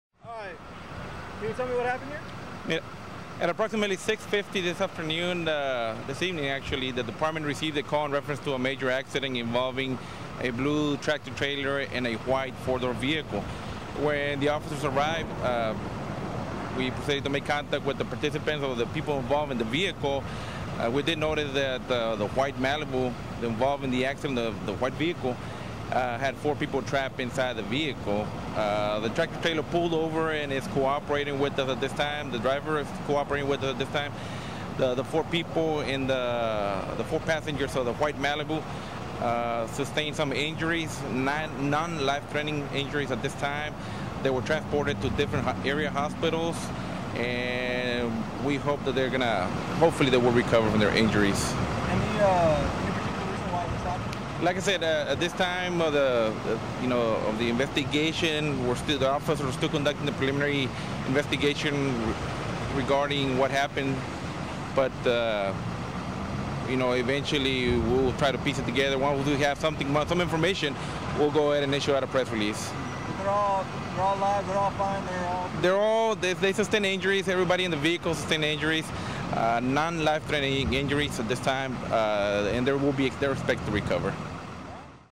RAW VIDEO: Car crushed by 18-wheeler
Authorities are investigating an accident where a car ended up being crushed by an 18-wheeler in San Benito.